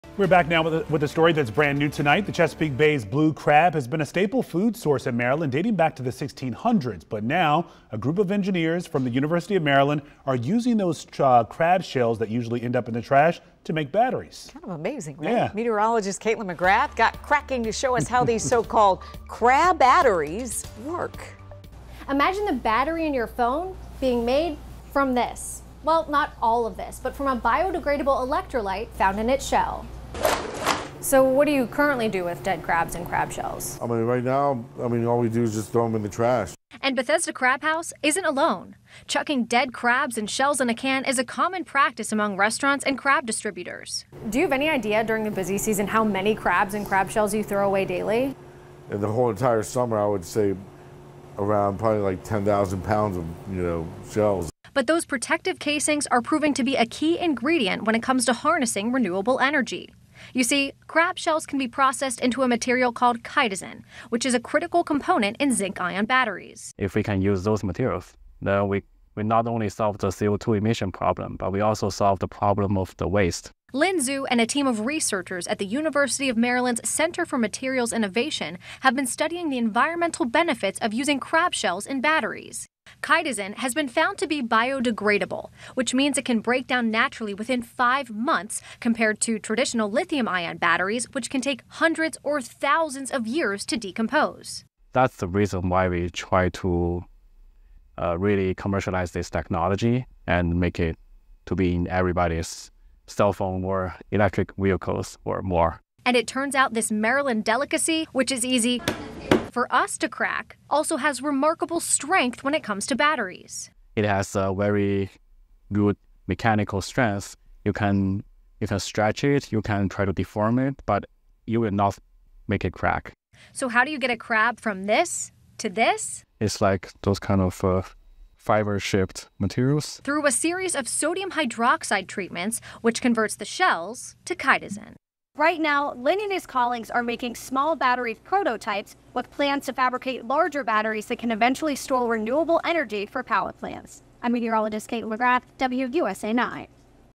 Turning crab shells into sustainable batteries
Imagine the battery in your phone being made from crab shells. A group of local engineers are using a biodegradable electrolyte found in the shell to make batteries.